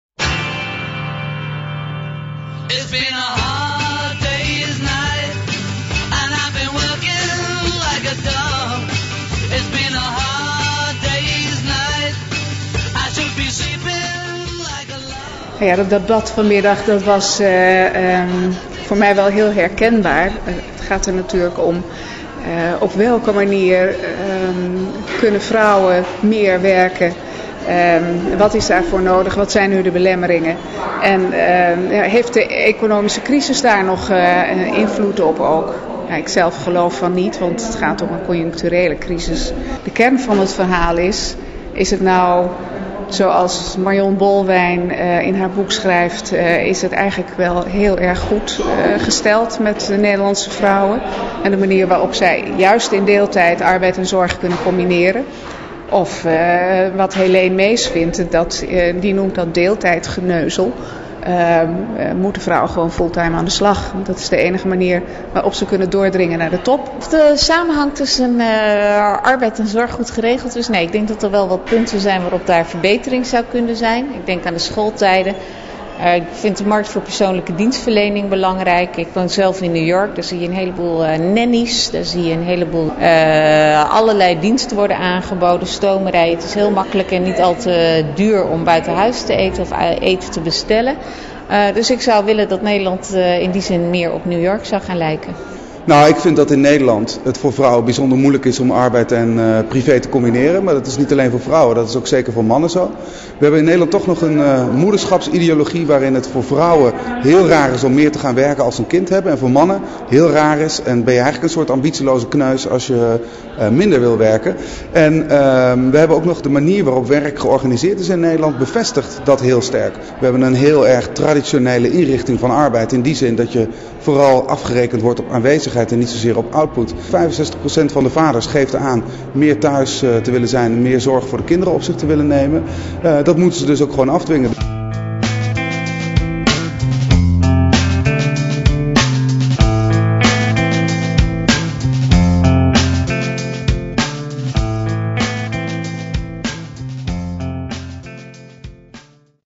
Opzij Café Vrouwen, mannen, werk en zorg.
VROUWEN, MANNEN & EMANCIPATIE: WIE DOET WAT?
Moeten alle vrouwen een betaalde baan? Hoe krijg je mannen aan het zorgen? Wie doet wat? En wat heeft de economische crisis hierop voor invloed? Het derde Opzij Café stond in het teken van internationale Vrouwendag en werd door Opzij gevierd in De Balie te Amsterdam.
Thema: Vrouwen, mannen, werk en zorg.

Pannelleden:
Pia Dijkstra (Taskforce Deeltijd Plus)
Rutger Groot Wassink (Papa Plus)
Heleen Mees (econoom, jurist en columnise Opzij)
Marjon Bolwijn (journaliste Volkskrant en auteur van De ideale werknemer is een moeder)